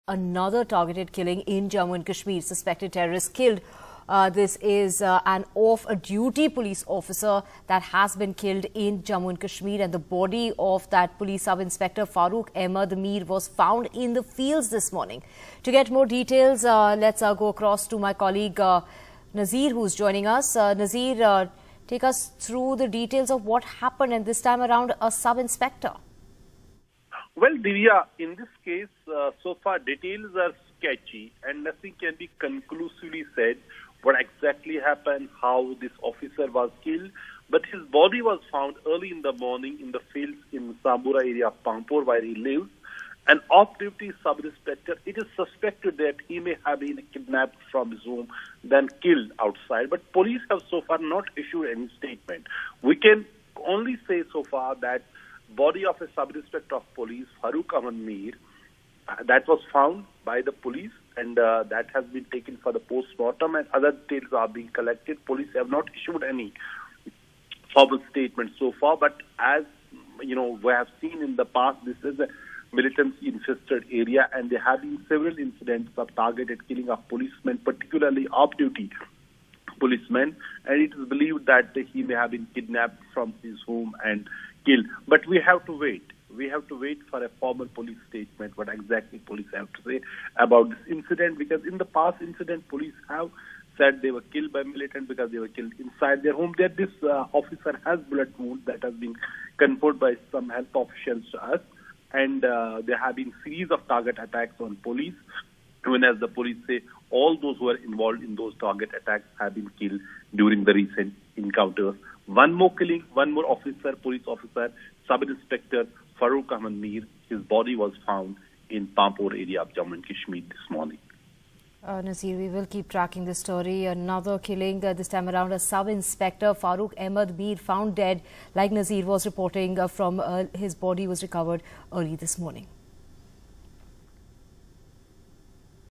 Police Officer Found Dead In Jammu And Kashmir's Pampore
An off-duty police officer was killed in Pampore area of Jammu and Kashmir. While a formal police statement is awaited, terrorists are suspected to have committed the act.